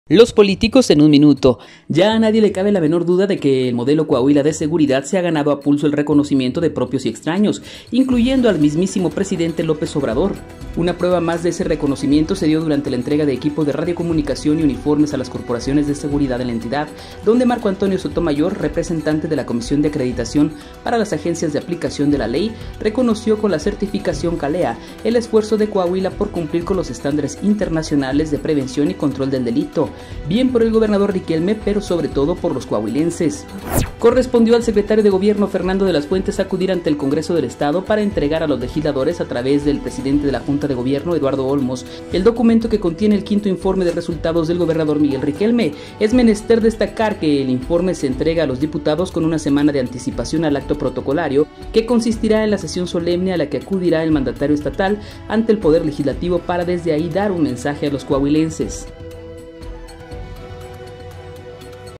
LOS POLÍTICOS - Un buen reconocimiento 🖊️
Ya a nadie le cabe la menor duda de que el llamado Modelo Coahuila en materia de seguridad, se ha ganado a pulso el reconocimiento de propios y extraños, incluyendo entre éstos al mismísimo presidente López Obrador…  de esto y mucho más en la columna LOS POLÍTICOS de EL DIARIO de Coahuila